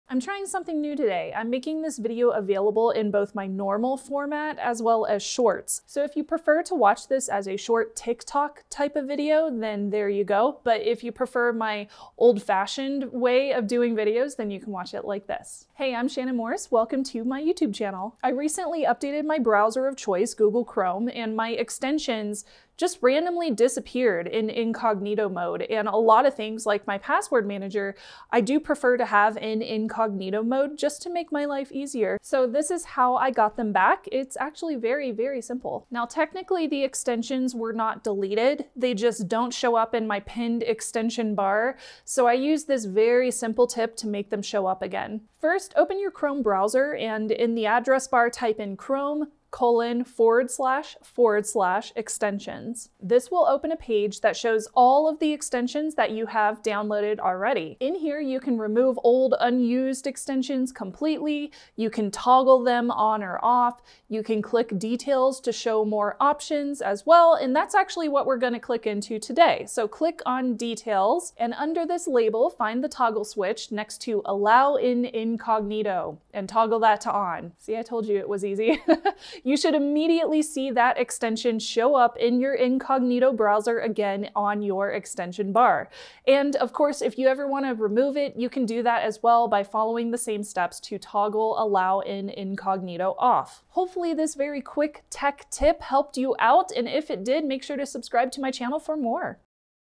Extensions Disappeared In Incognito Mode On Google Chrome
Did your extensions disappear from Incognito Mode in Google Chrome? This is how you can get them back in less than a minute!